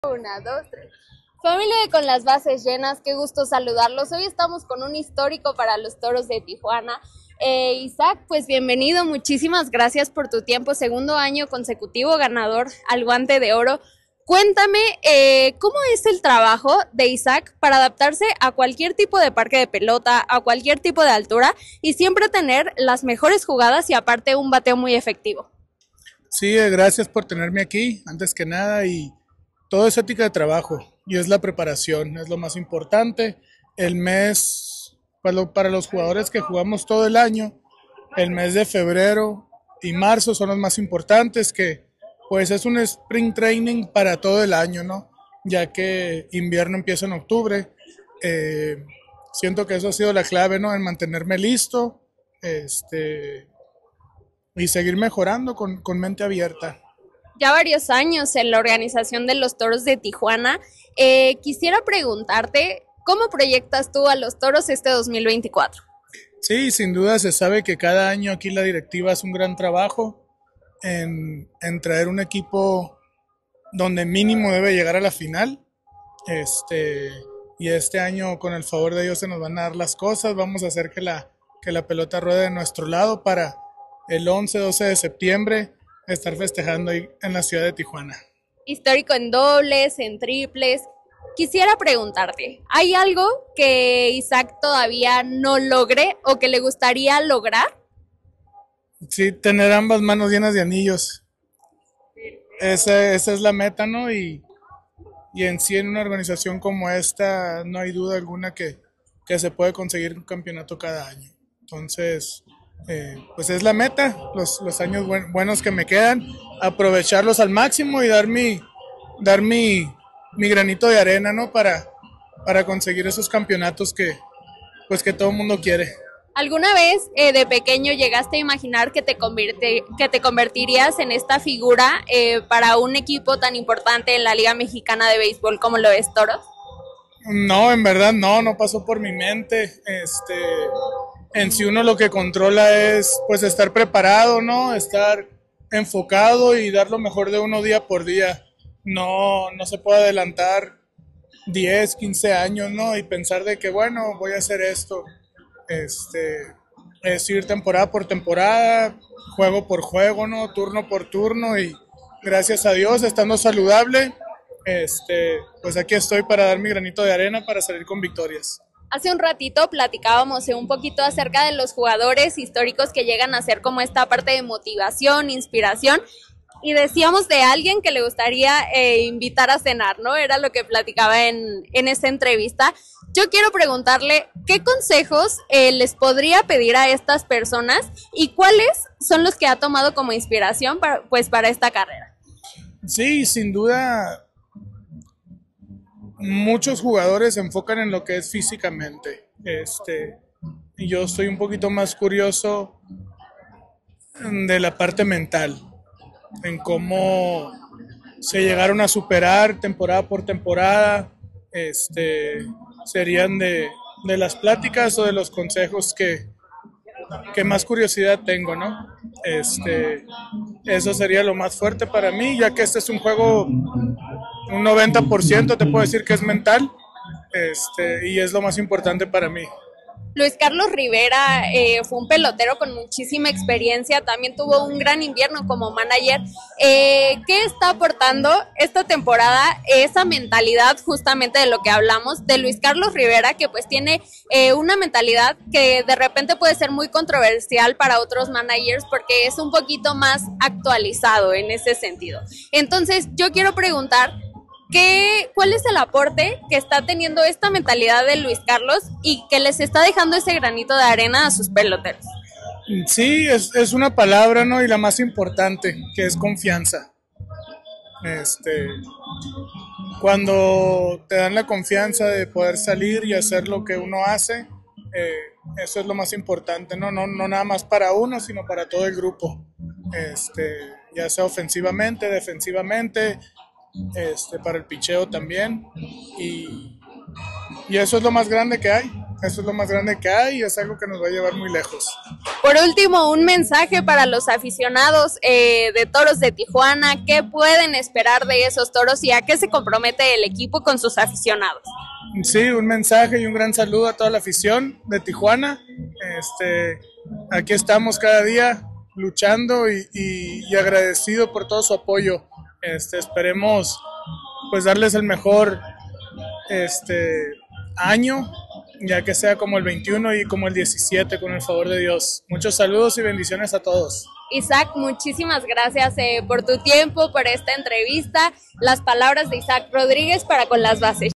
ISAAC RODRÍGUEZ y su GUANTE de ORO en LMP y BRILLANDO en MLB con TOROS
¡Isaac Rodríguez habla sobre su segundo título consecutivo en LMP al Guante de Oro! 🔥⚾️